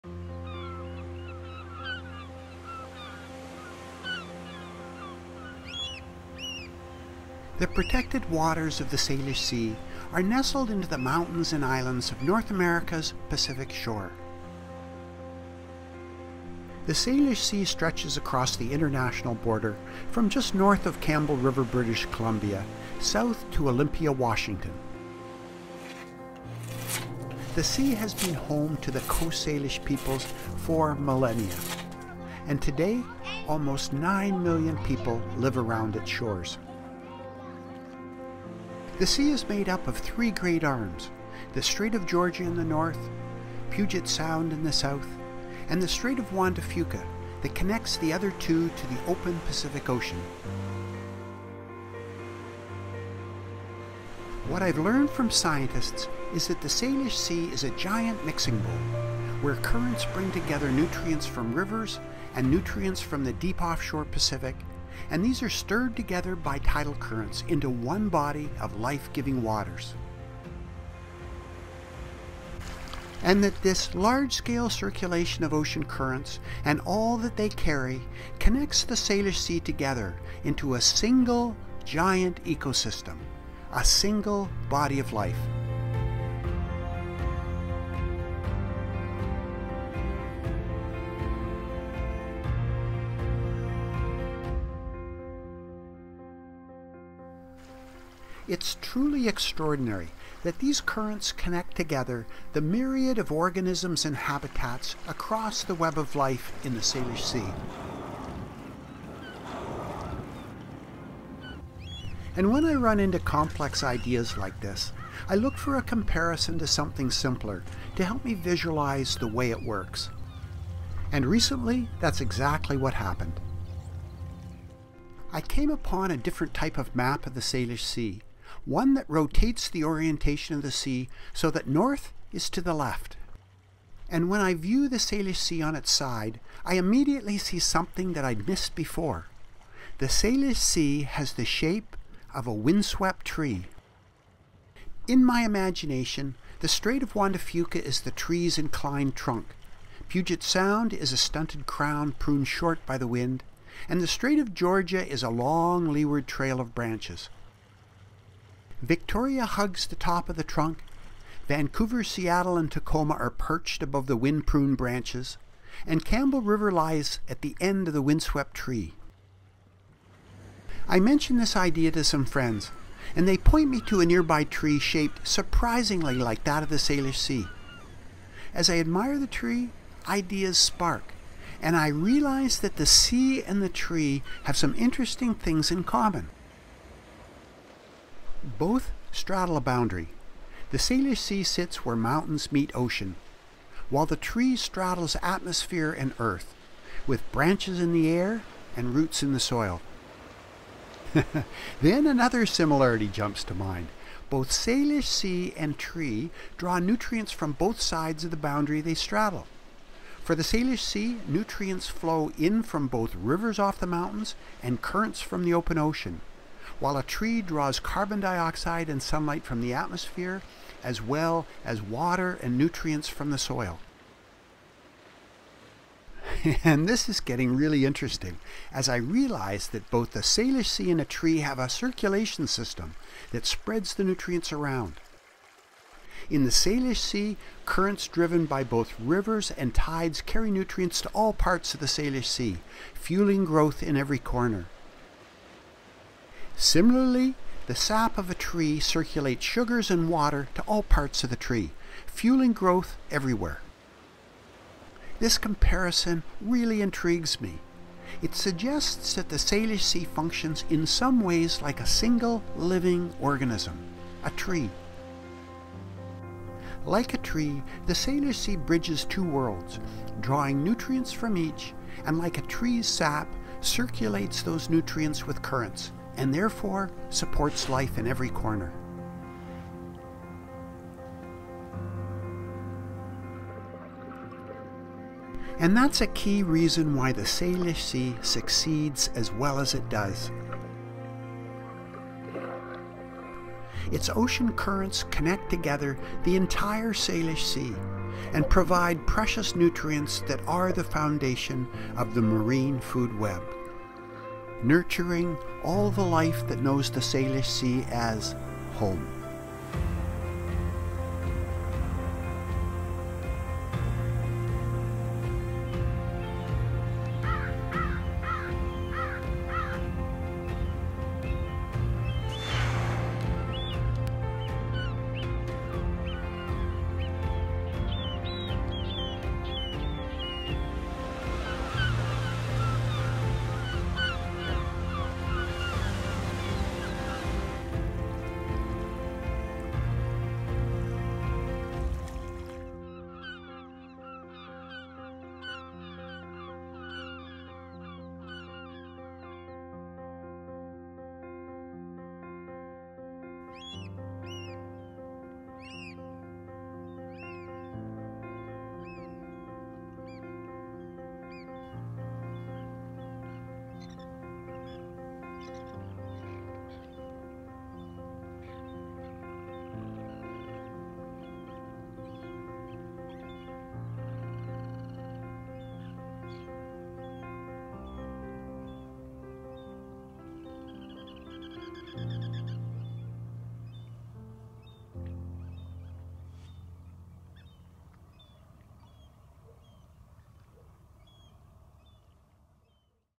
The Salish Sea - A Single Body of Life
The way that the Salish Sea functions as a single ecosystem has similarities to a single living organism, a tree.